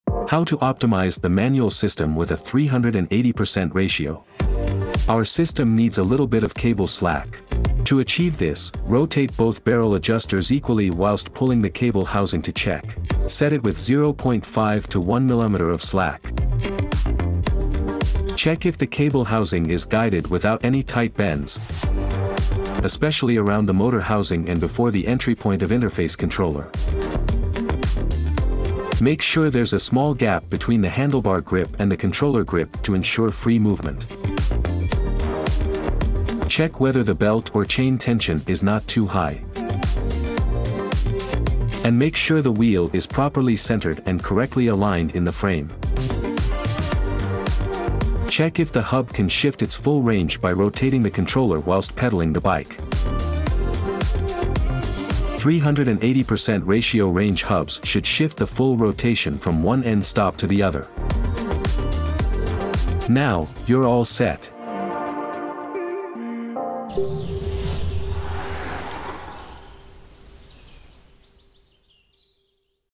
How to optimize the manual system with a 380% ratio
00:04 - Our system needs a little bit of cable slack.
To achieve this, rotate both barrel adjusters equally whilst pulling the cable housing to check.
Set it with 0.5 to 1mm of slack.

00:17 - Check if the cable housing is guided without any tight bends, especially around the motor housing and before the entry point of the interface controller.

00:27 - Make sure there's a small gap between the handlebar grip and the controller grip to ensure free movement.

00:35 - Check whether the belt or chain tension is not too high.

00:40 - And make sure the wheel is properly centred and correctly aligned in the frame.

00:47 - Check if the hub can shift its full range by rotating the controller whilst pedalling the bike.

00:54 - 380% ratio range hubs should shift the full rotation from one end stop to the other.

01:02 - Learn more on how to optimize the manual system with 380% ratio at